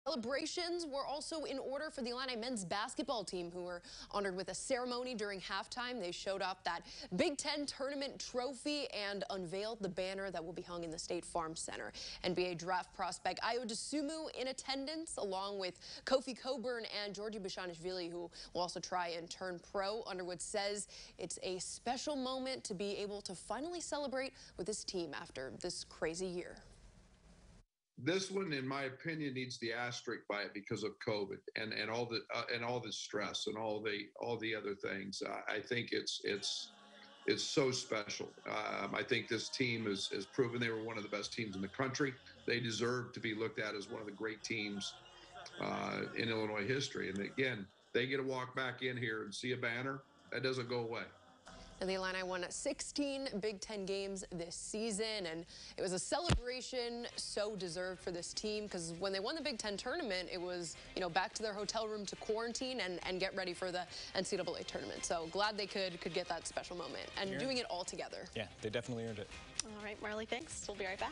Big Ten Tournament Championship banner unveiled, Illini celebrate season
CHAMPAIGN (WCIA) -- Illinois Men's Basketball was recognized at the Orange and Blue Spring game on Monday night, to unveil the Big Ten Tournament Championship banner that will be hung in the State Farm Center.